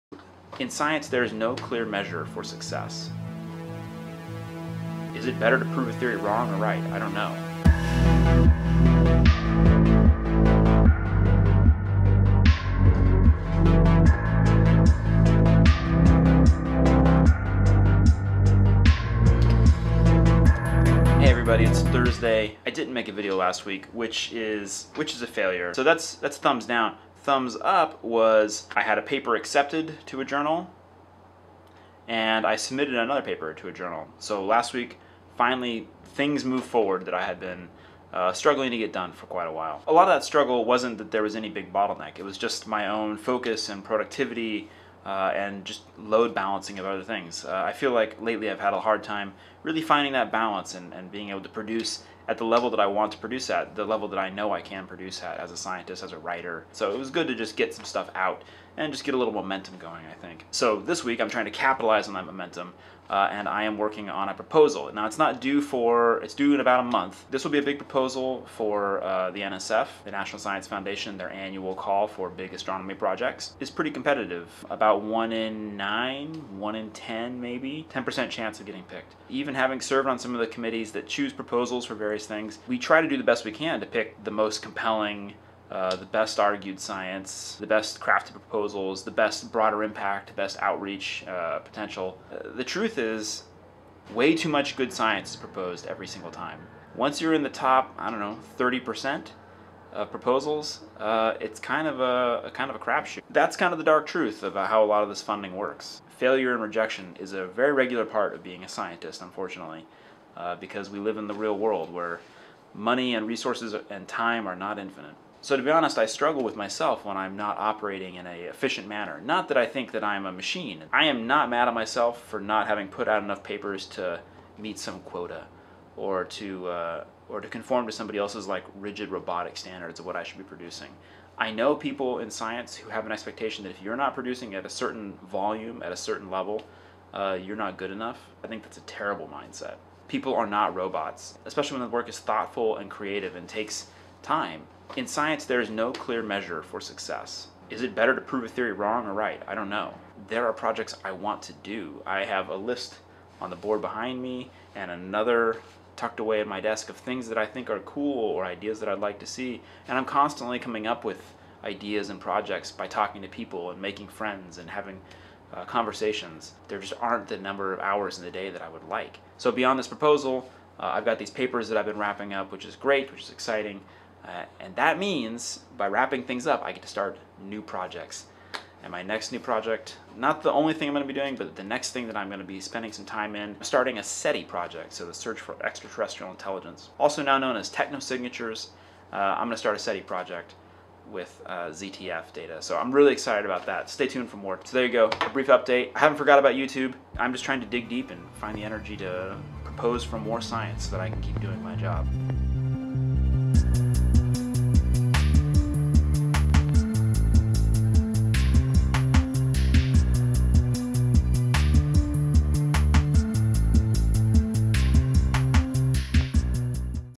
FUN(ding proposals)
Last week I managed to get 1 paper submitted and 1 paper accepted to a journal. WOOOO! This week I'm trying to capture that momentum and write a funding proposal. Wish me luck...

== ASTRO VLOG: Episode 40 ==

GEAR
My vlogging camera: Nikon D7000
My main lens: Sigma 17-50mm f/2.8
My mic: Takstar SGC-598